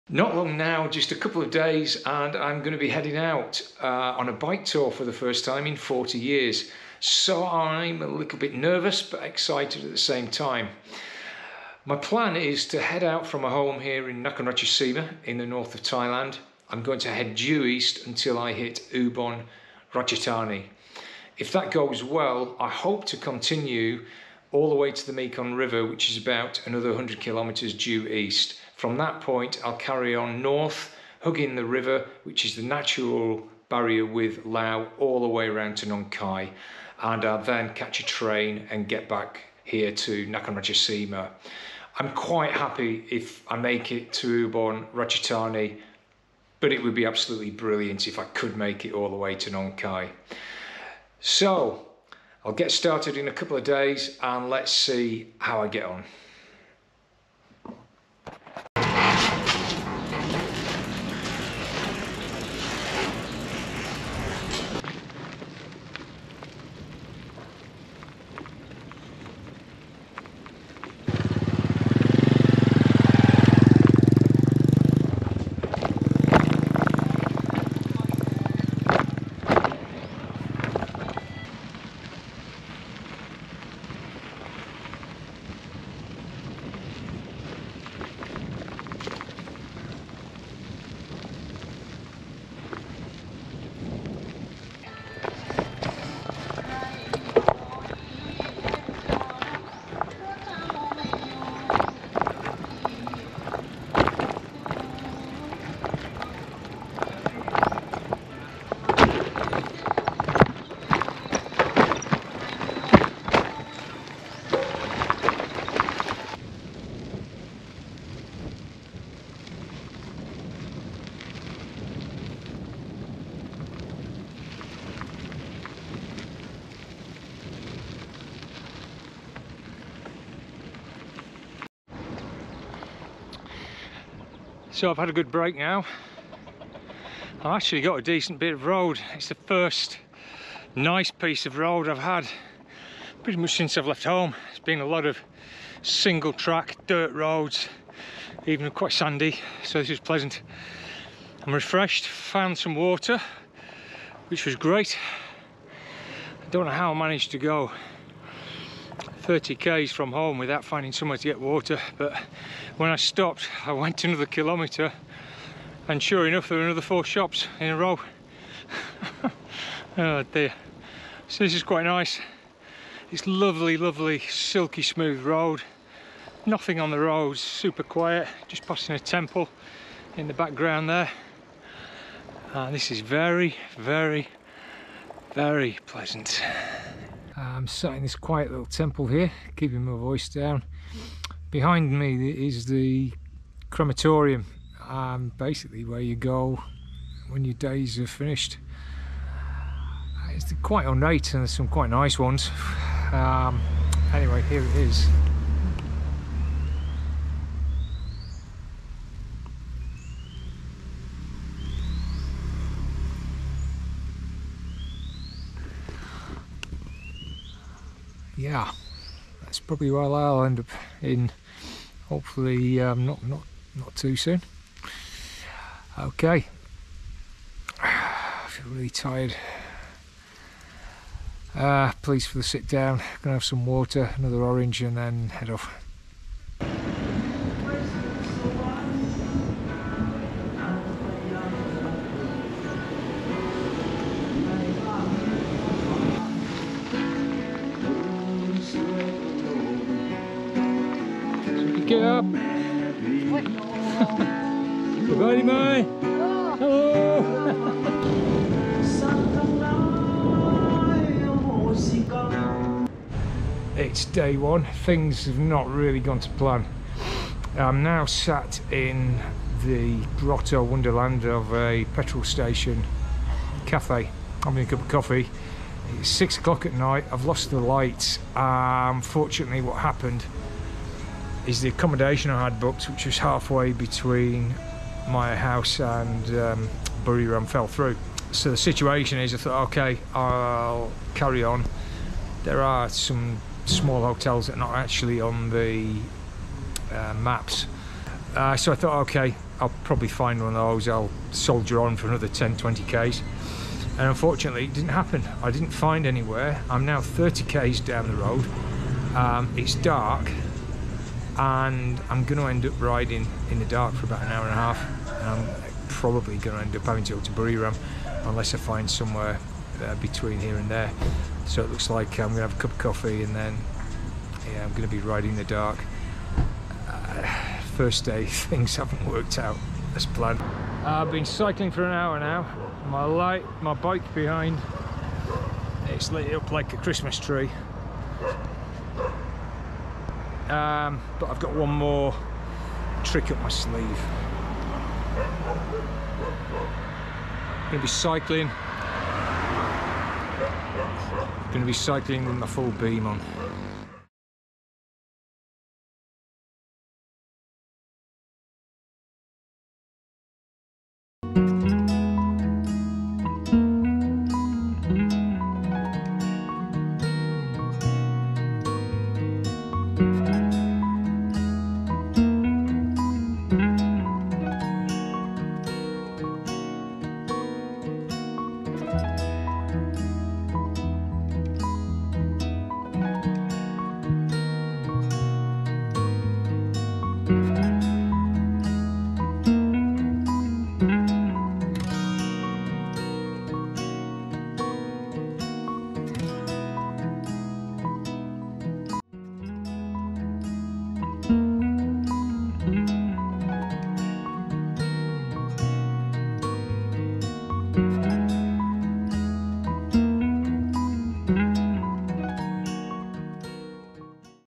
Thailand: Northern Bike Tour - Through the Rice Fields to Buriram
This is the first day of my tour across Isaan, in the north of Thailand, heading east to the Mekong river. On the first leg of the journey, I set out from my home in Korat, (Nakhon Ratchasima) hoping to make it to Buriram in two days. It's the first time in over forty years I've strapped on a set of panniers to go for a tour. I'm excited to see how things have changed!

0:00 Route Summary
0:58 The Set-Off
1:11 The Old Bridge
2:11 Ride Review
3:07 Resting in a Temple
4:23 Revised Plan
5:30 Cycling in the Dark
5:53 Arrival in Buriram and Night Market